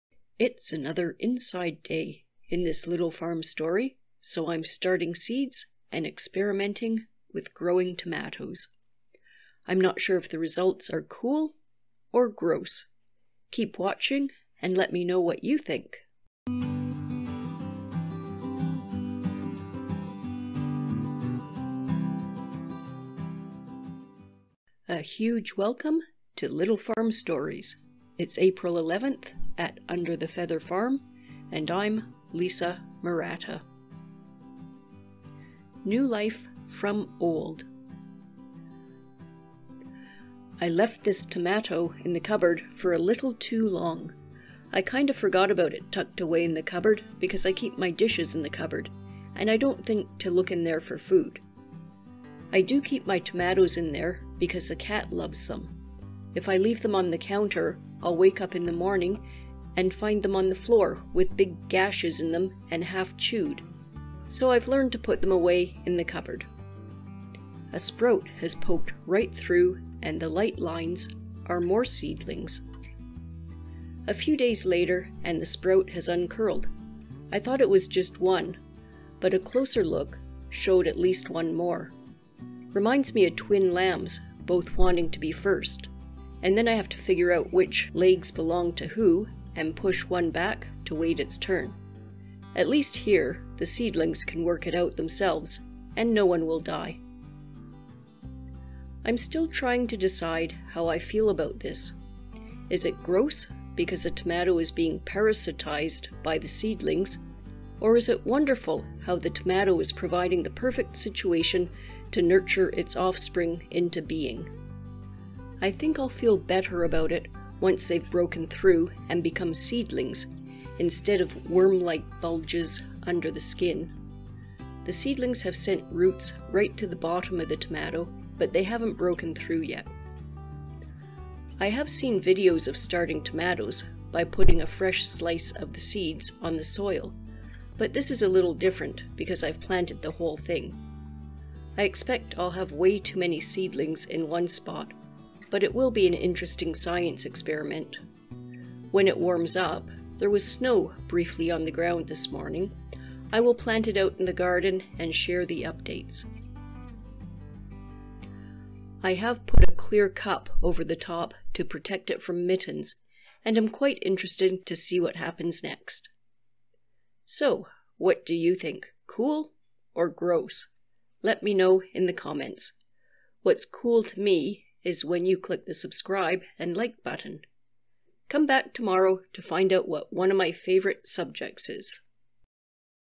Growing TOMATO SEEDLINGS in a Tomato - April 11 Little Farm Stories
Today's Little Farm Story is my experiment to find out if you can grow tomato seedlings right in the tomato, courtesy of my cat.
Follow the gentle farm life at my small farm in south western British Columbia in these Little Farm Stories.